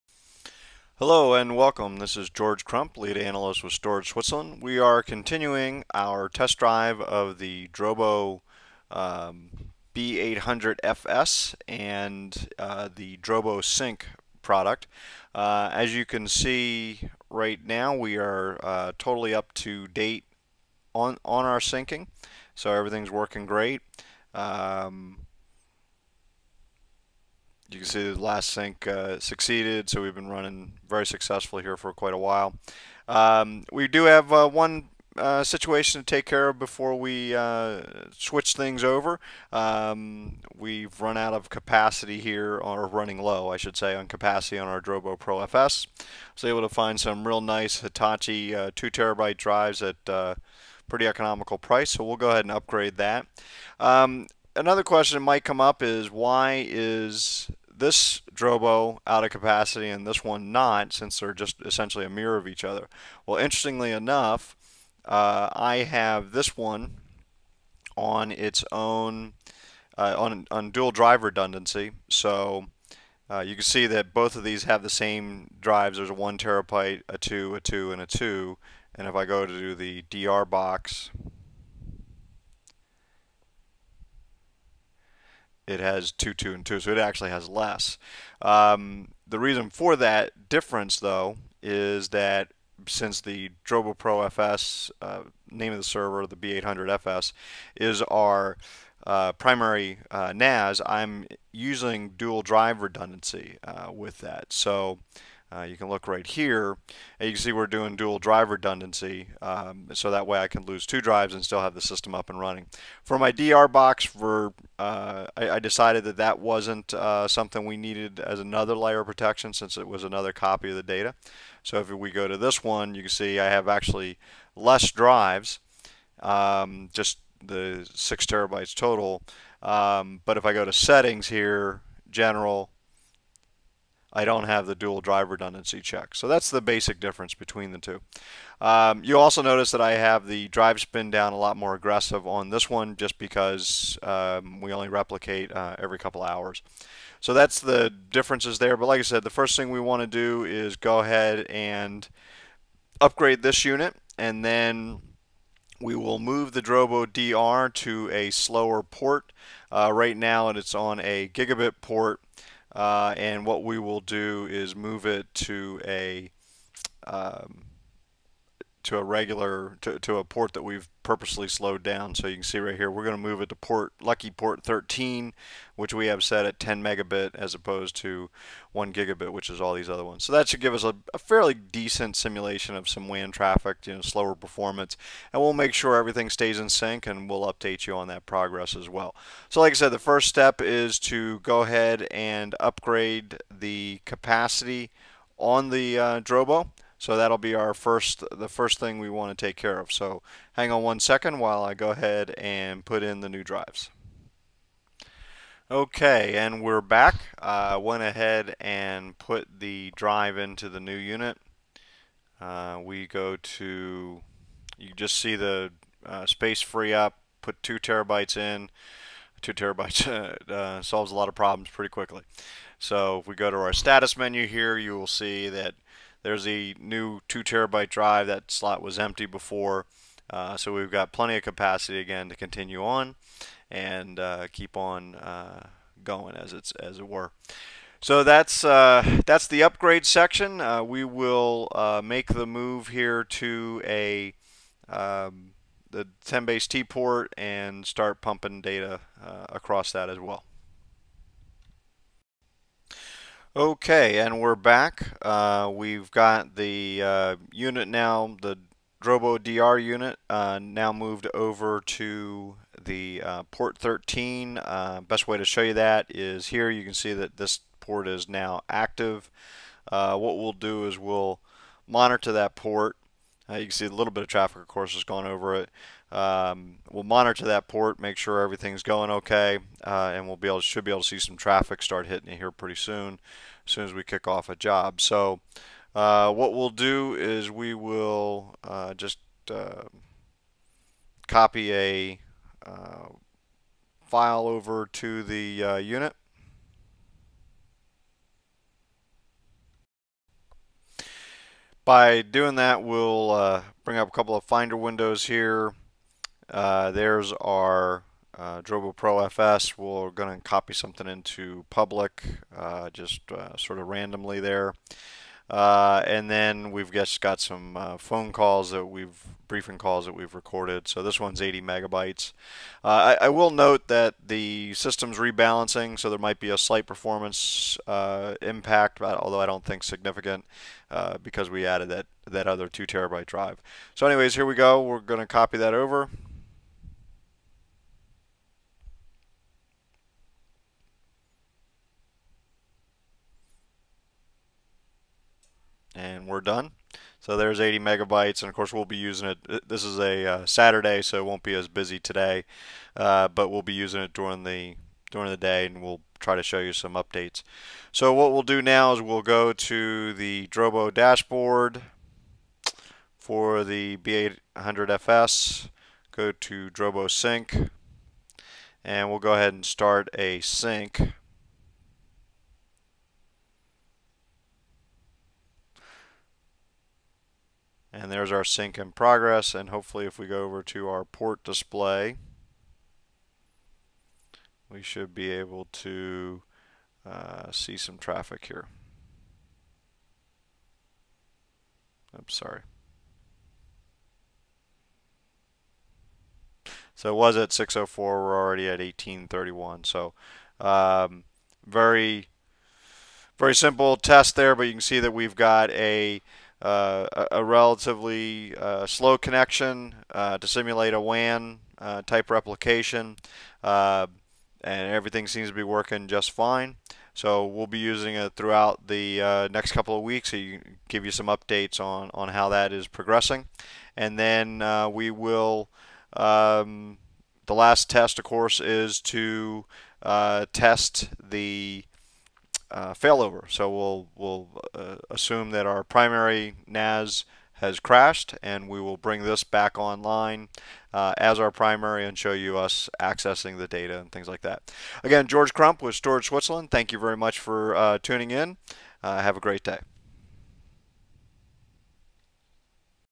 DroboSync10MB
Storage Switzerland's ongoing test of Drobo's DrobSync DR feature for the Drobo B800fs product line. This update features a drive upgrade and then a move to a 10mb port to simulate a WAN connection